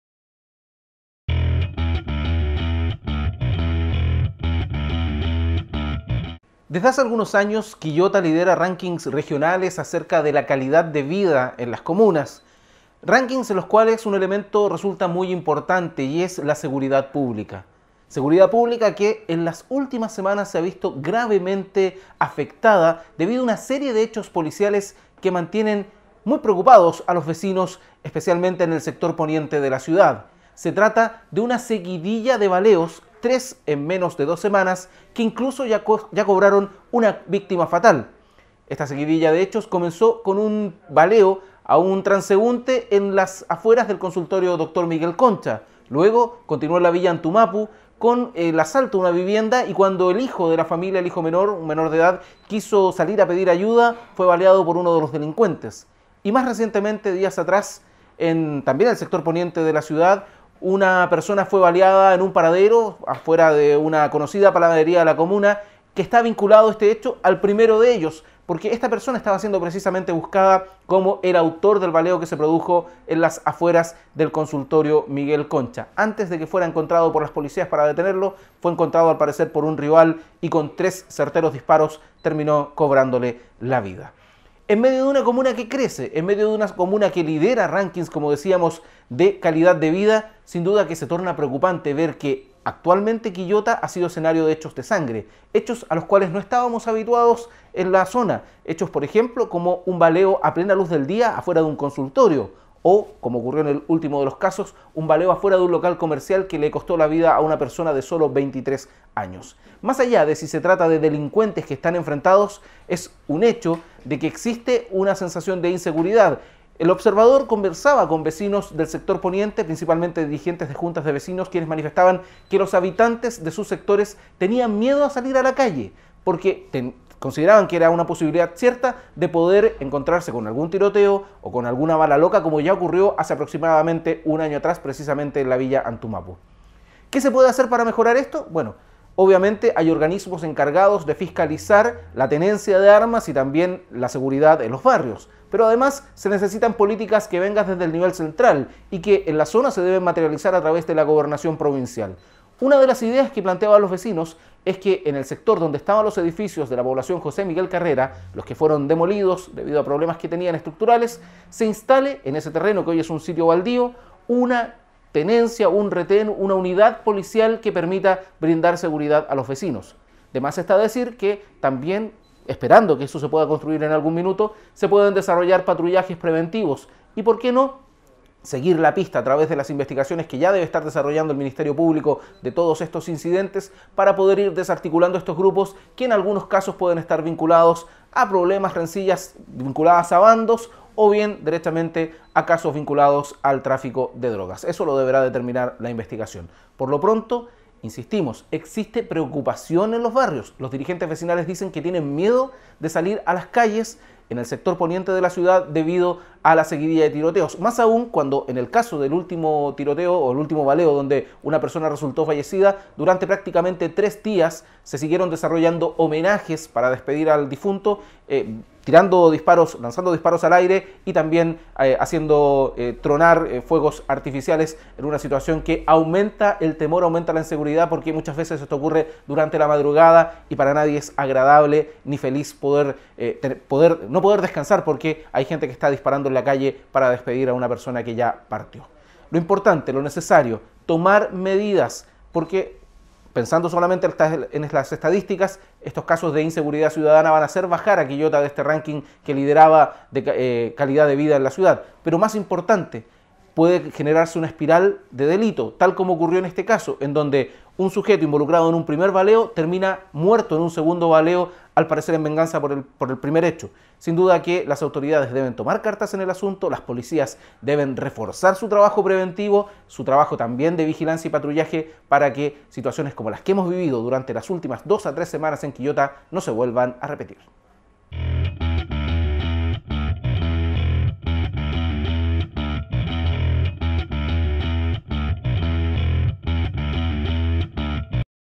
Gustavo Rodriguez - Tiroteos en Quillota y la pérdida de la calidad de vida
El jefe de informaciones, Gustavo Rodríguez Catalán, analiza los recientes hechos de violencia que han ocurrido en Quillota, entre ellos, una seguidilla de baleos, algunos a plena luz del día, que han dejado dos heridos graves y una persona fallecida. Esto afecta gravemente la calidad de vida de los habitantes, en una ciudad que lidera rankings regionales por el bienestar que ofrece.